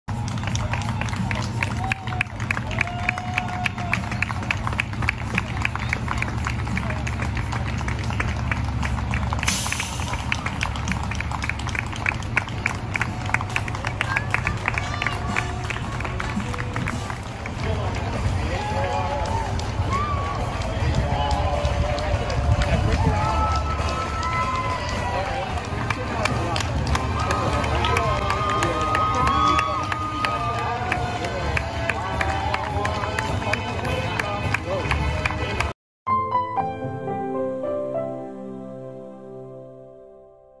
Citizens clap for medical workers in NY
(ECNS)-- In the video taken outside of Mount Sinai Hospital in New York, citizens and firefighters are seen clapping for the healthcare workers.